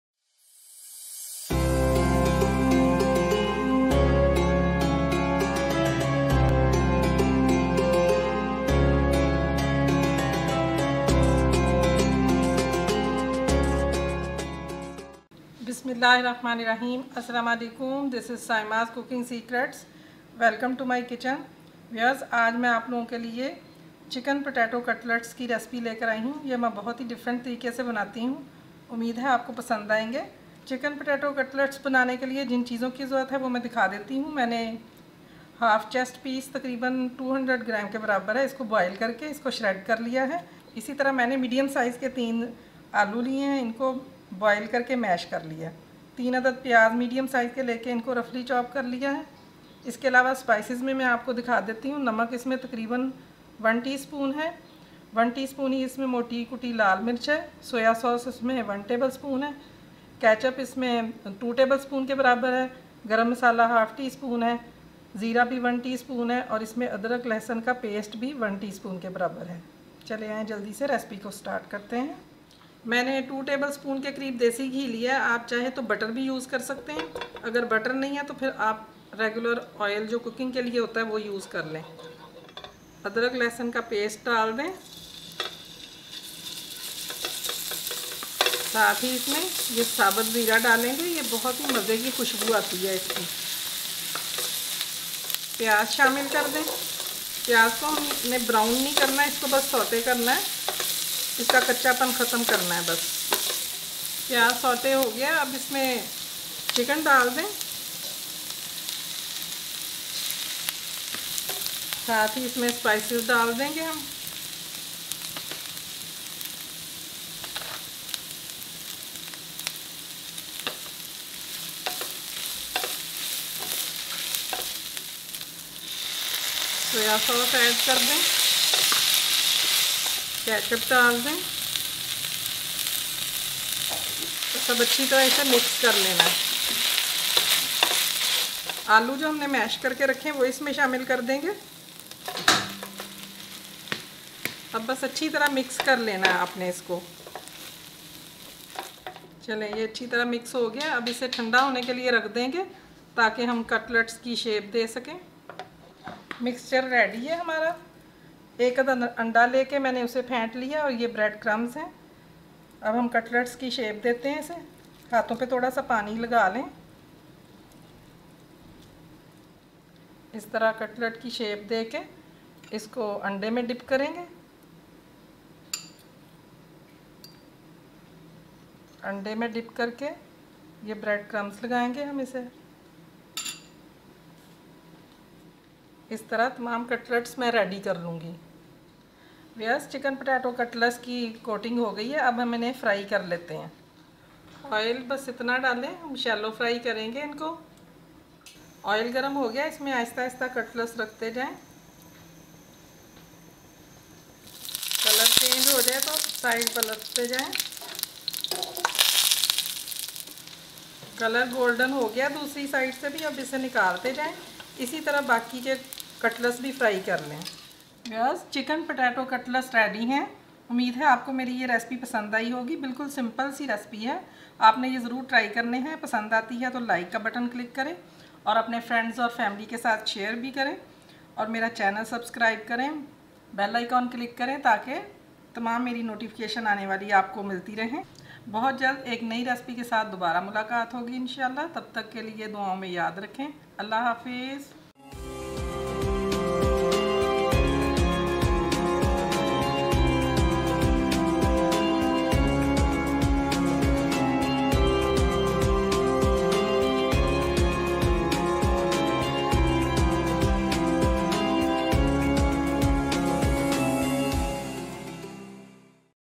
Spicy Chicken Cutlets - Ramadan Special Crispy Chicken Potato Cutlets
Spicy chicken potato recipe, this is a Ramadan Special recipe, make sure to try this homemade crispy chicken potato cutlets. This recipe of cutlets is very easy to make.

Ingredients For Spicy Chicken Cutlets

Boiled Boneless Chicken 200 grams
Boiled Mashed Potato x 3
Onion x 3
Garlic and Ginger Paste 1 Tsp